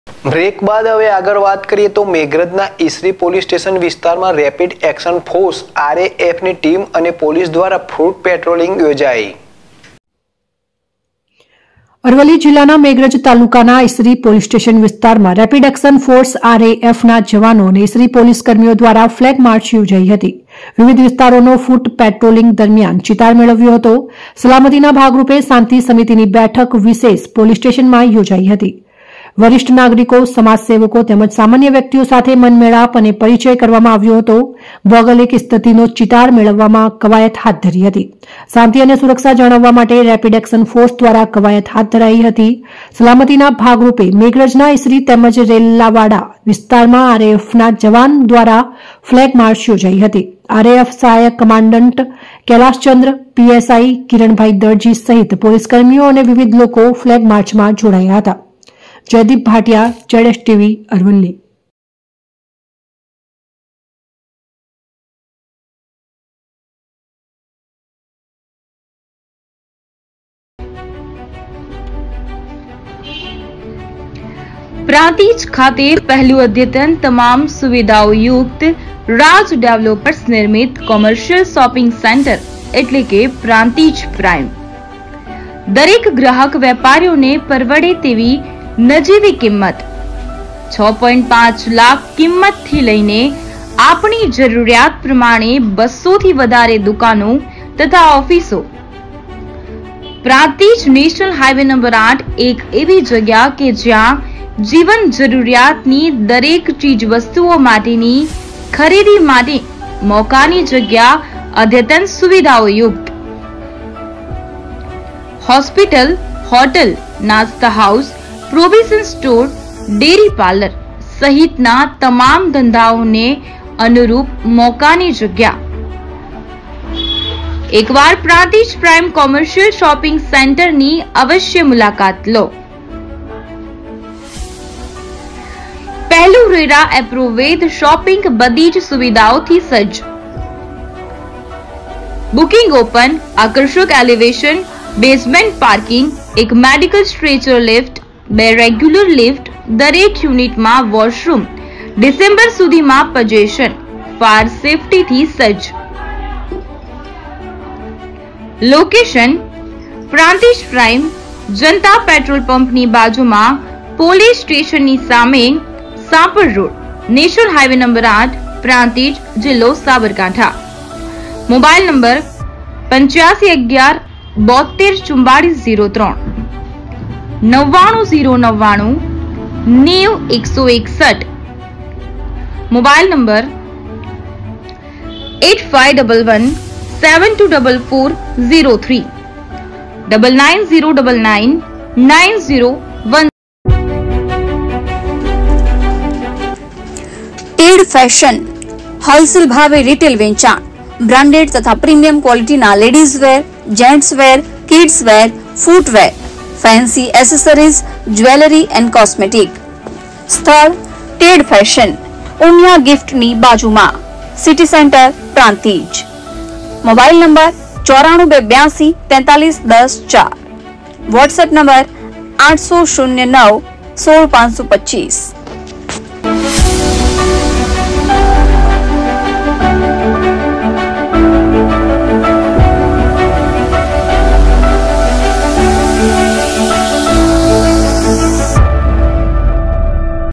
મેઘરજના ઇસરી પોલિસ સ્ટેશન વિસ્તારમાં રેપીડ એક્શન ફોર્સ RAF ની ટીમ અને પોલીસ ધ્વારા ફુટ પેટોલિંગ
મેઘરજ ના ઇસરી પોલિસ સ્ટેશન વિસ્તારમાં રેપીડ એક્શન ફોર્સ RAF ની ટીમ અને પોલીસ ધ્વારા ફુટ પેટોલિંગ યોજાઈ